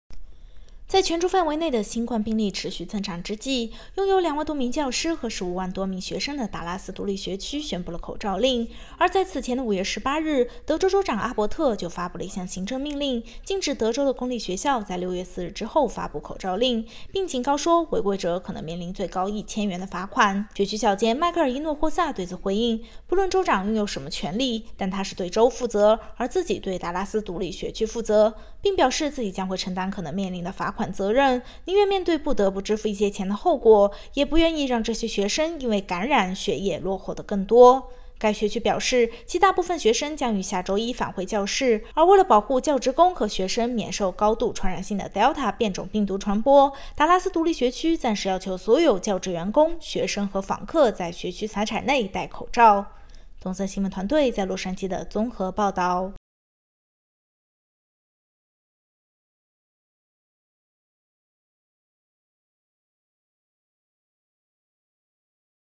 不顧州長行政命令 達拉斯獨立學區頒布口罩令
德州第二大學區宣布，要求學生、教職員工至少從新學年開始以後在校園內戴口罩，此舉違反了德州州長格雷格阿博特發布的行政命令。

在全州範圍內的新冠病例持續增長之際，擁有2萬多名教師和15萬多名學生的 達拉斯獨立學區周一宣布了口罩令，而在此前的5月18日，德州州長阿博特 就發布了一項行政命令，禁止得州的公立學校在6月4日之後發布口罩令，並警告說，違規者可能面臨最高1,000元的罰款。

學區校監邁克爾伊諾霍薩对此回应，不管州長擁有什麼權力，但他是對得州負責，而自己對 達拉斯獨立學區負責，並表示自己將會承擔 可能面臨的罰款 責任，寧願面對不得不支付一些錢的後果，也不願與讓這些學生因為感染學業落後得更多。

該學區表示，其大部分學生將於下週一返回教室，而為了保護教職員工和學生免受高度傳染性的Delta變種傳播，達拉斯獨立學區 暫時要求所有教職員工、學生和訪客在學區財產內戴口罩。